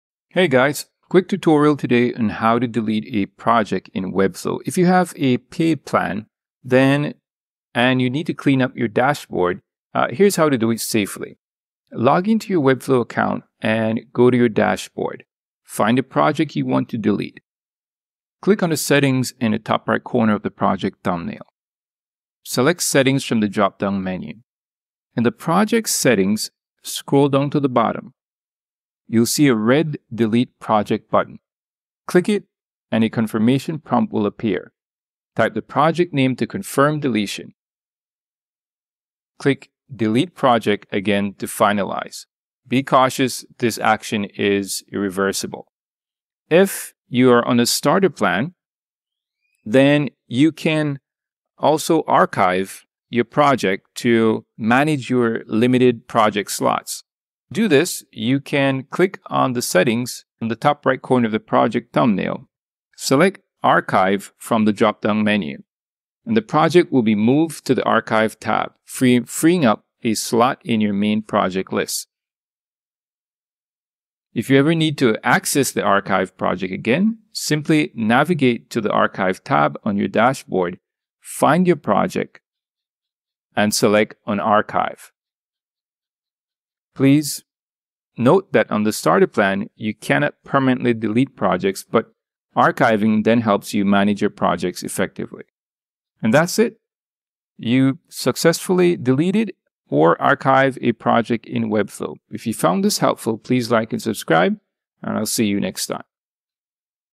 How to Delete a Webflow Project Webflow Tutorial (Step By Step)
How to Delete a Webflow Project Webflow Tutorial

In this tutorial, I'll walk you through how to delete or archive a project in Webflow. Whether you're on a paid plan or a starter plan, you'll learn the steps to safely manage your dashboard and free up project slots.

Deleting or Archiving a Project in Webflow
How to Manage Your Webflow Projects Effectively
Step-by-Step Guide to Project Deletion in Webflow